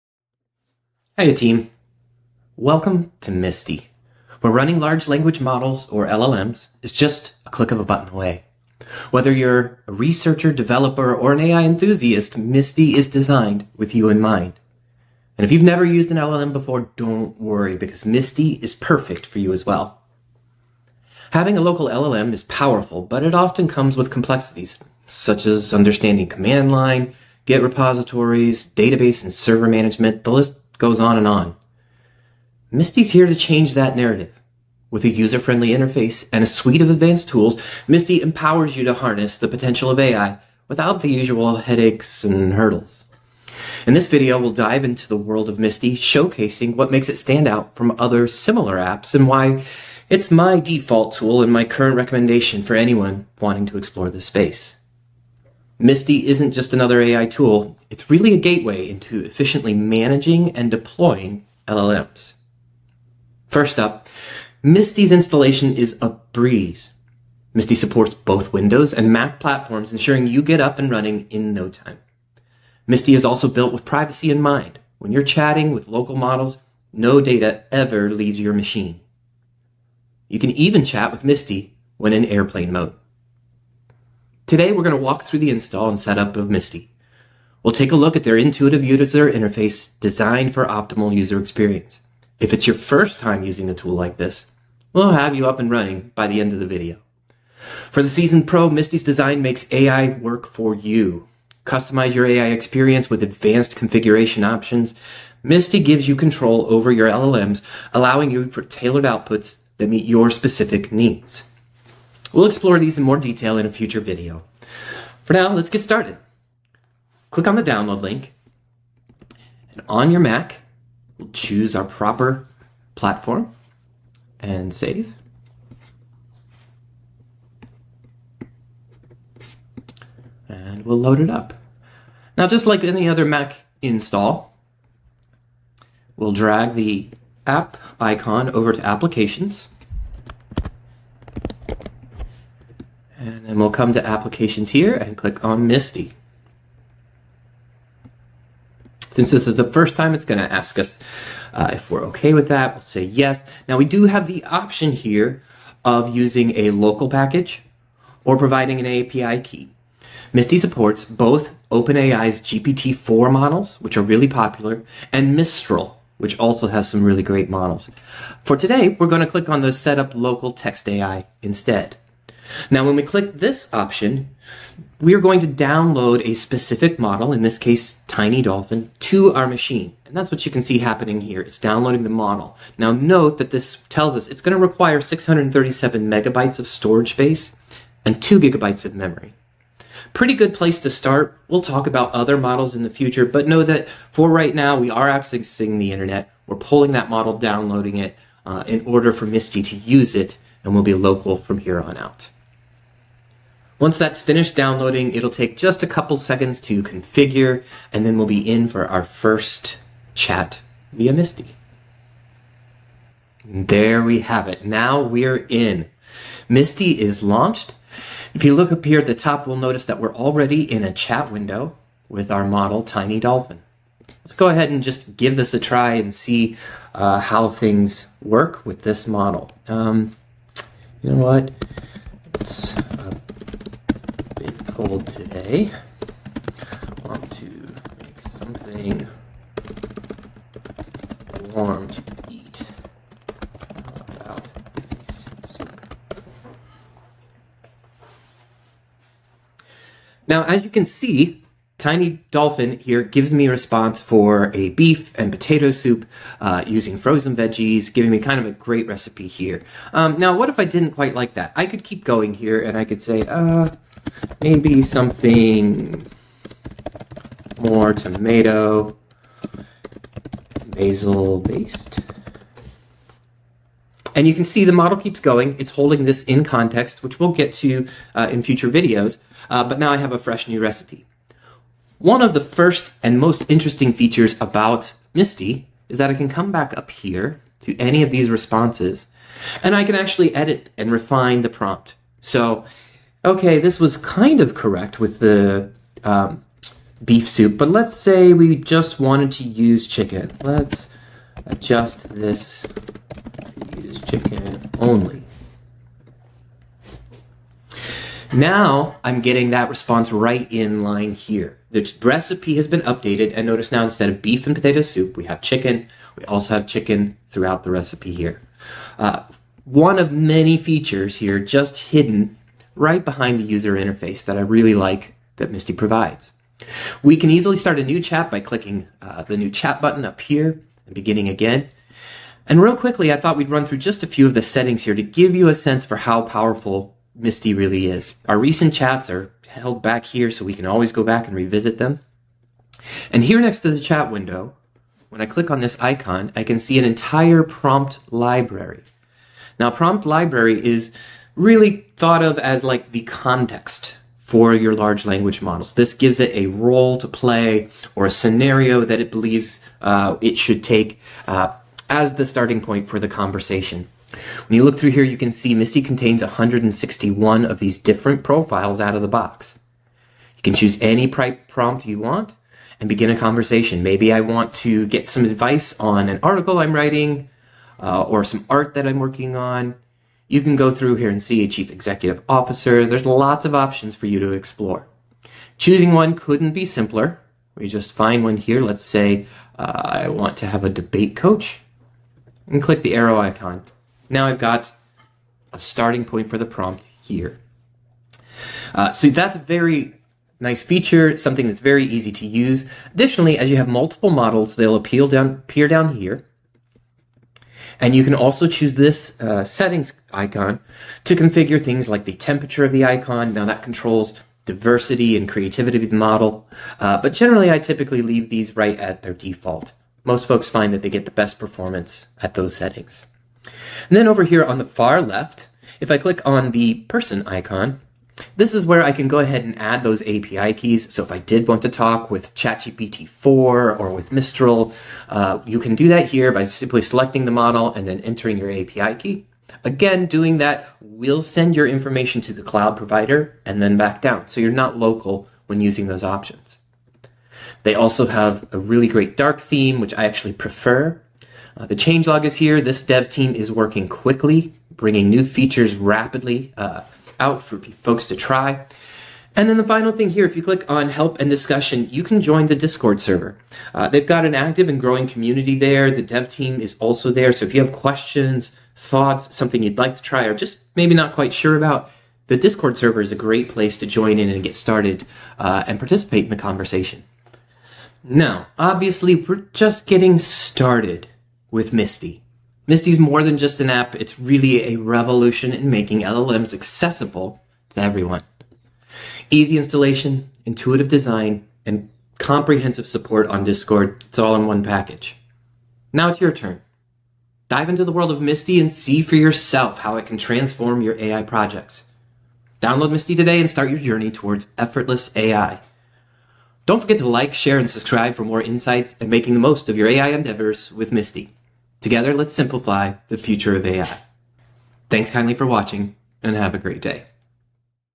Msty App - An LLM for Everyone
In this video, we'll walk through the installation and setup of my new favorite app for experimenting with Large Language Models (LLMs). This app is perfect for beginners and advanced users a like. Get started with Generative AI on your local machine for a private experience - your data is never shared.